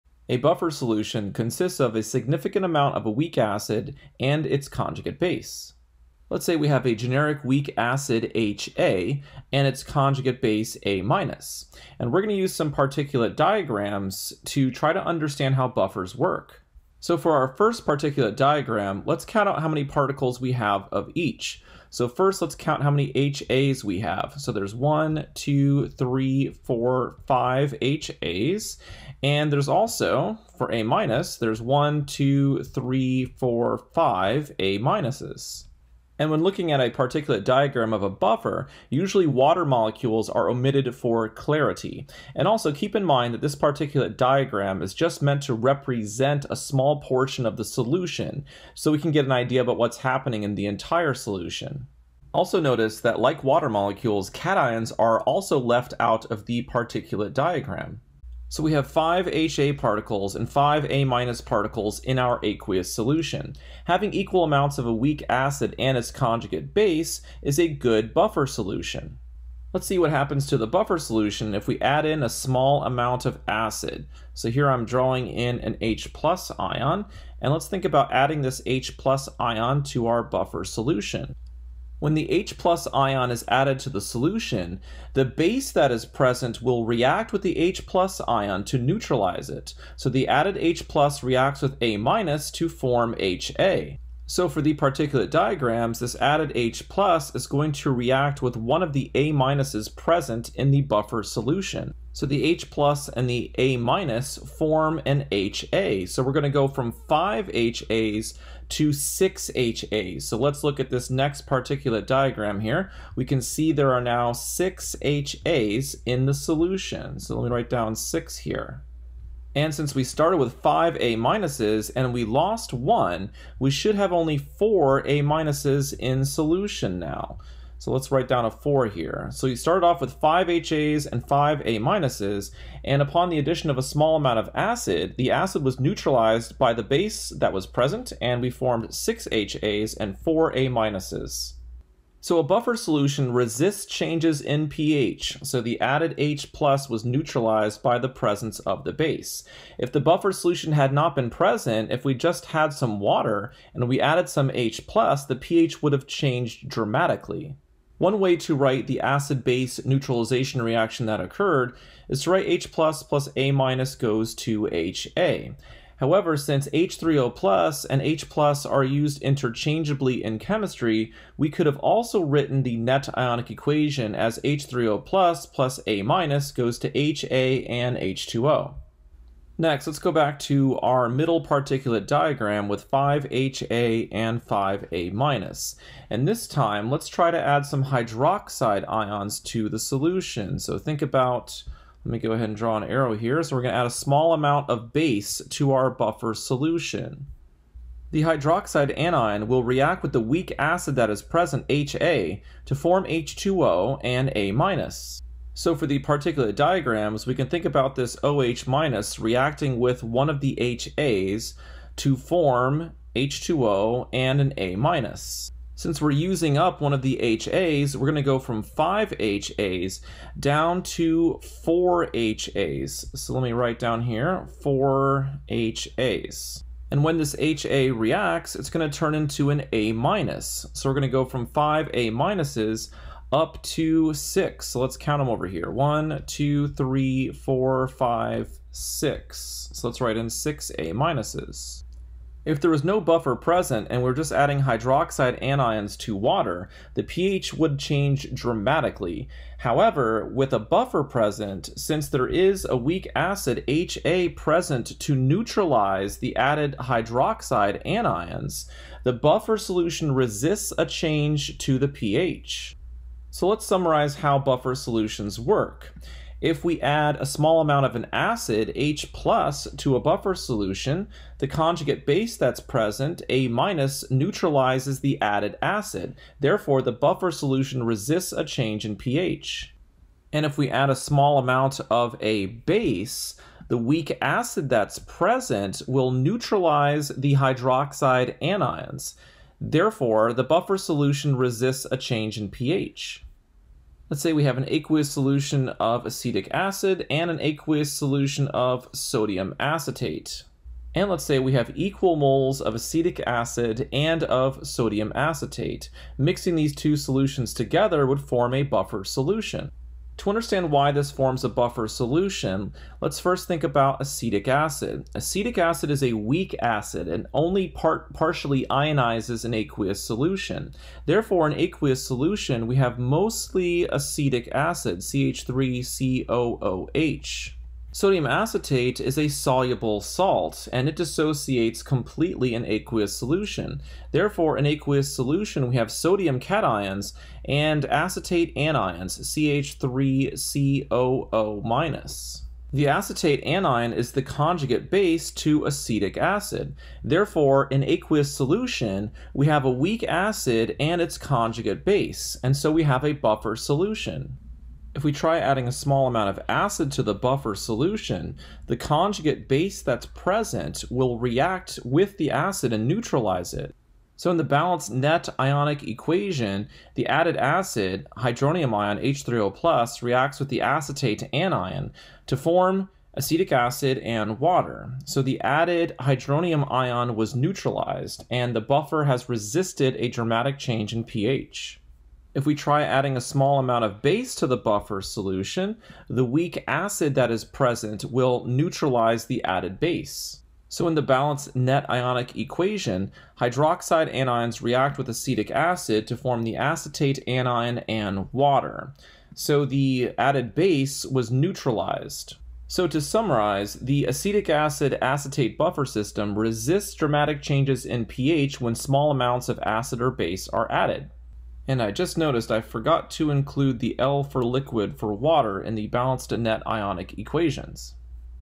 Properties of buffers | Acids and bases | AP Chemistry | Khan Academy
Buffer solutions contain high concentrations of both a weak acid and its conjugate base (or a weak base and its conjugate acid). Because these components can neutralize added H⁺ or OH⁻, buffers are highly resistant to changes in pH. View more lessons or practice this subject